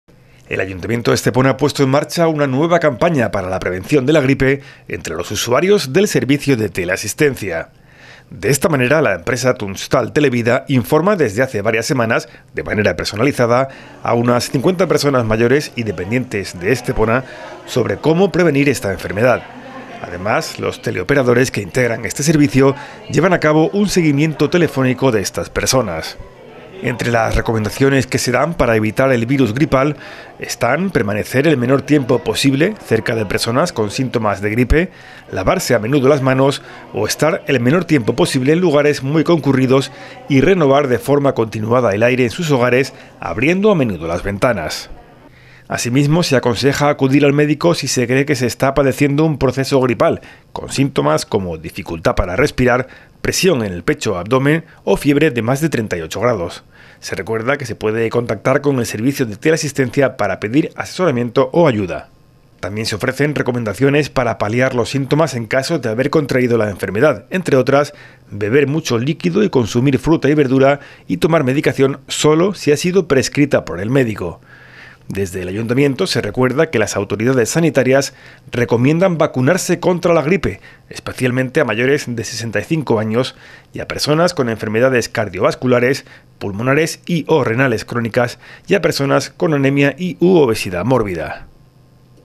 Campaña para la prevención de la gripe entre los usuarios del Servicio de Teleasistencia
El Ayuntamiento de Estepona ha puesto en marcha una nueva campaña para la prevención de la gripe entre los usuarios del Servicio de Teleasistencia. Con esta iniciativa se informa personalmente a medio centenar de personas mayores y dependientes